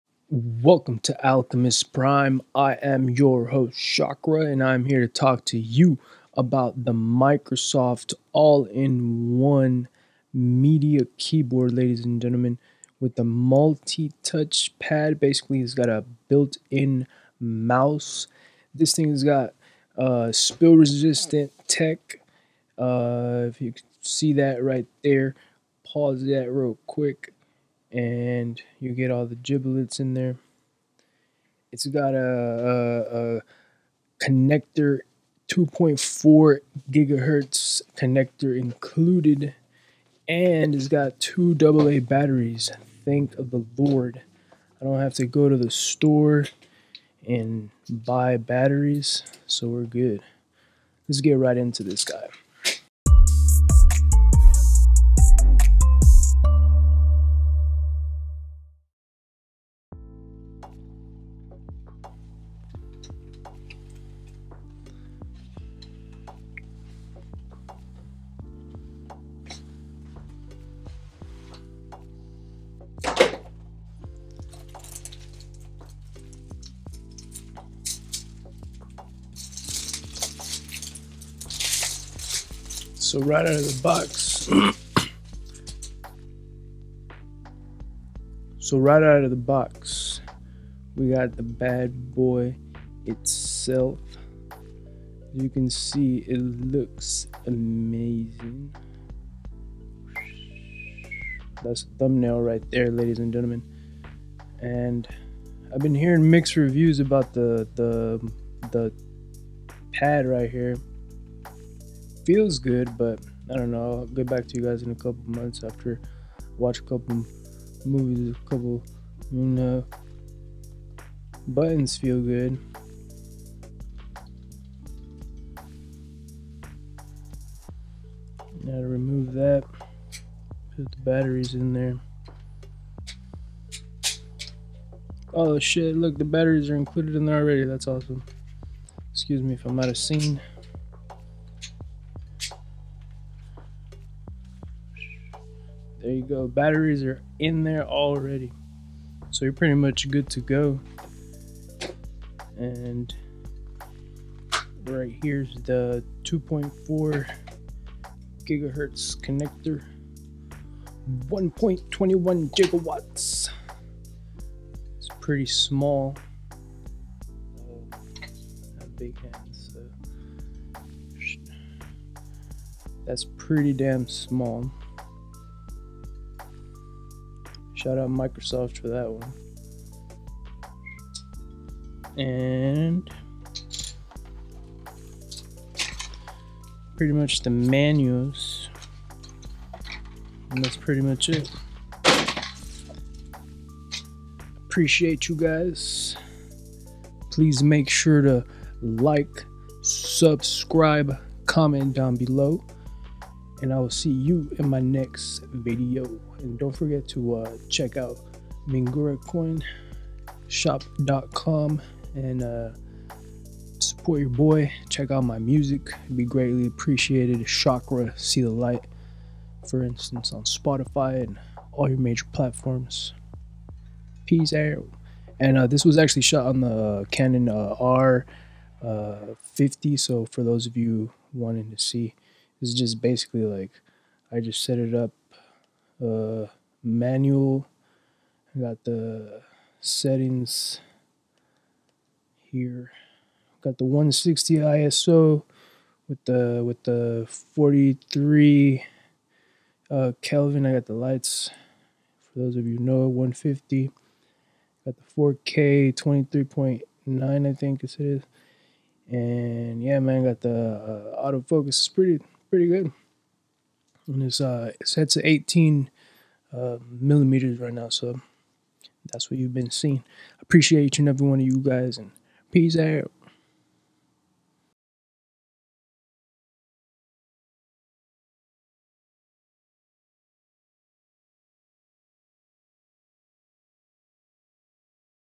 The Best! Microsoft All-in-One Media Keyboard (Unboxing) Shot On Canon r50 Sound On Mixpre-3 ii/SM7B
What's up, everybody?! Well, it's not so new, but will it stand the test of time? I like things that transcend and I'll see if I like the built-in mouse haptic keyboard and do a review later. Peace Ooout!